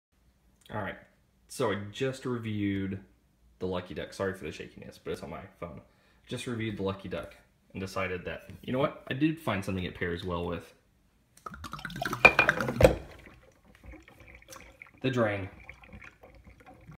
Follow-up: Lucky Duck Cabernet Sauvignon
I found this while editing another video. I have a follow-up to my review of the Lucky Duck Cabernet Sauvignon.